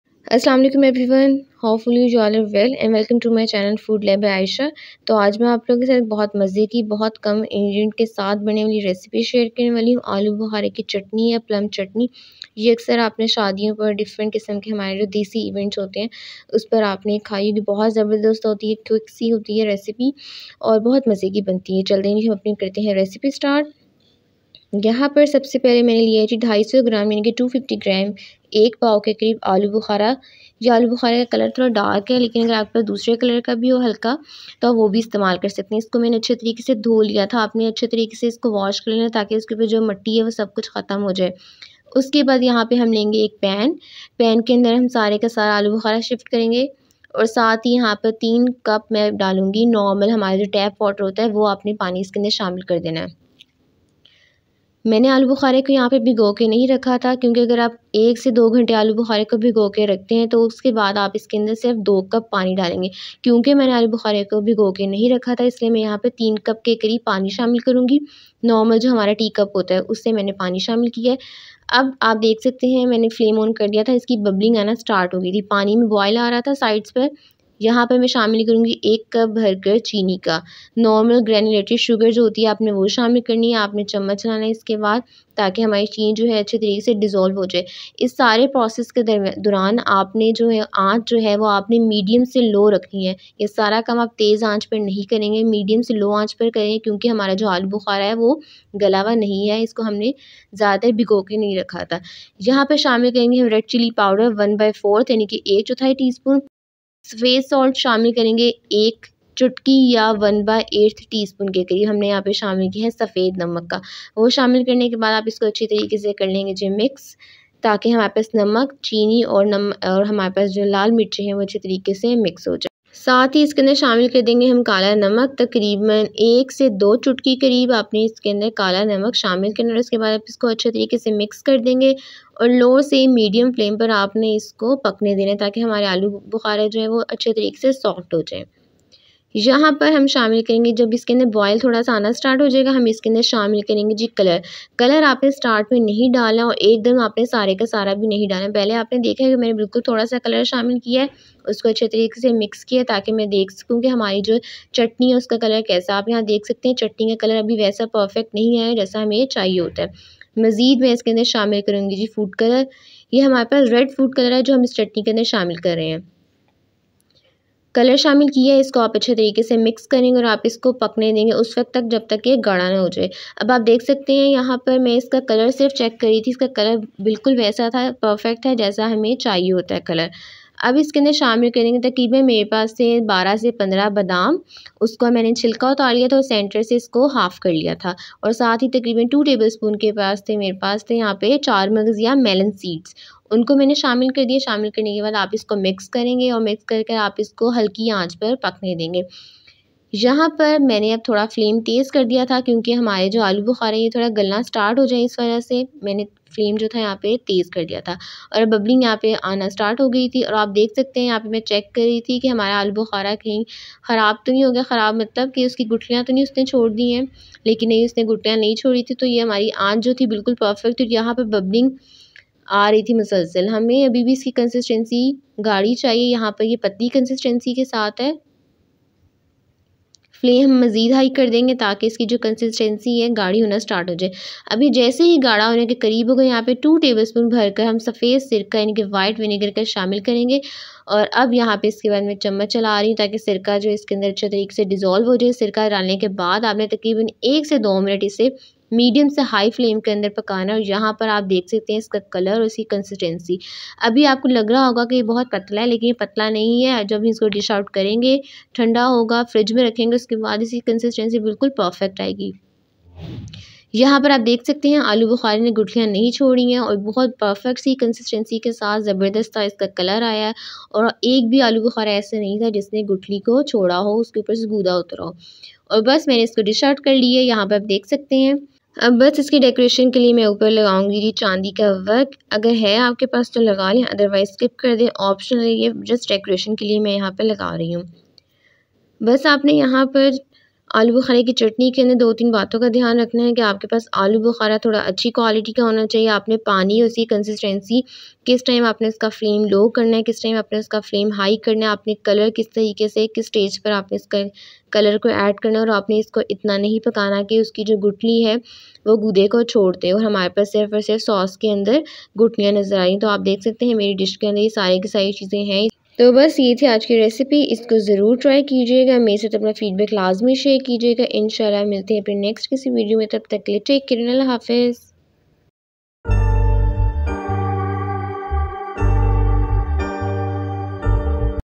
Aloo Bukhara ki Chatni || Plum Sauce || Shadiyon wali chatni
INGREDIENTS:
Aloo bukhara 250 g
Salt 1/8 tsp or 2 pinch
Black Salt 1 pinch
Melon seeds 2 tbsp
Almonds 13-14
White Vinegar 2 tbsp

⭐ Follow us on Snack app